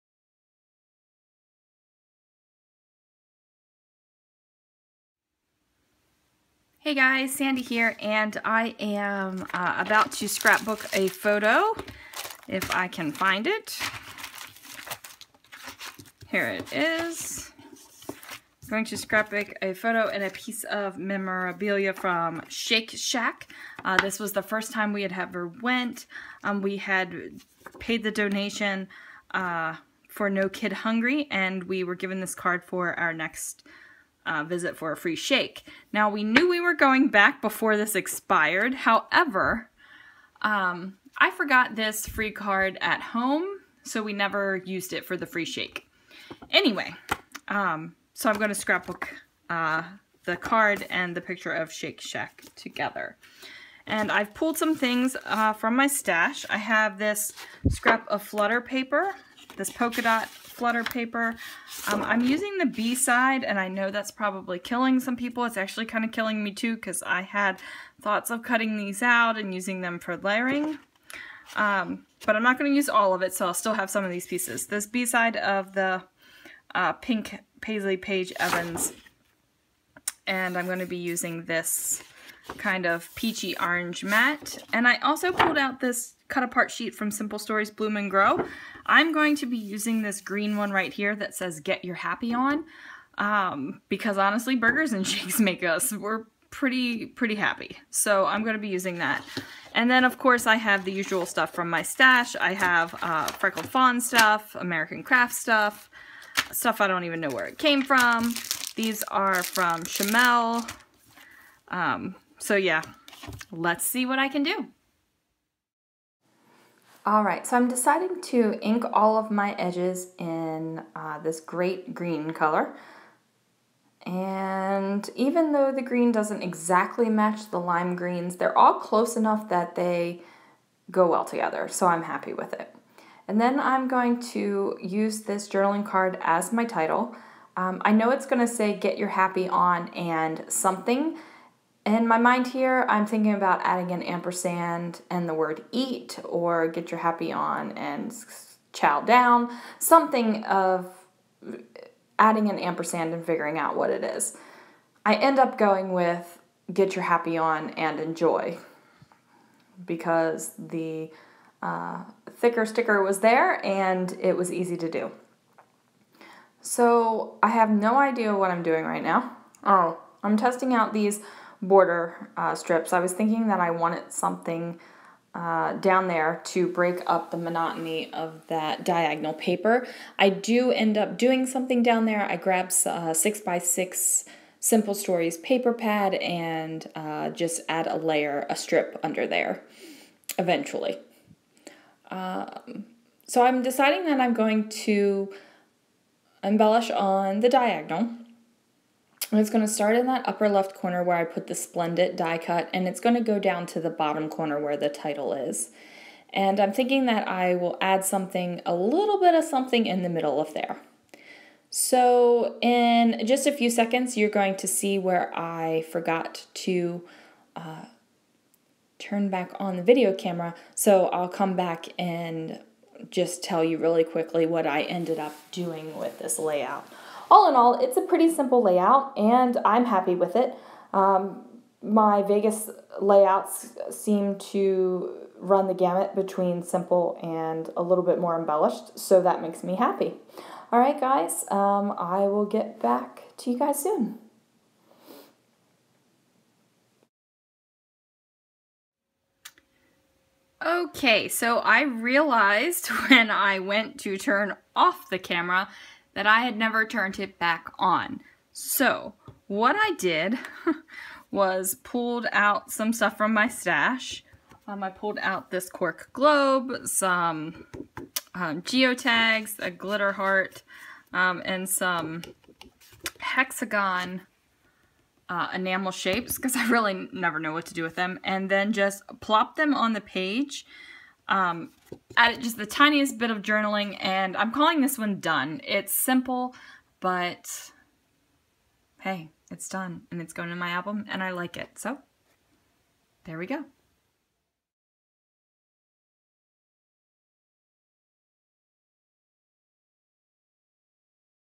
Scrapbook Process: Enjoy
Quick and Easy page from me today. Didn't do a whole lot of embellishing, but I am happy with the "simple" sometimes.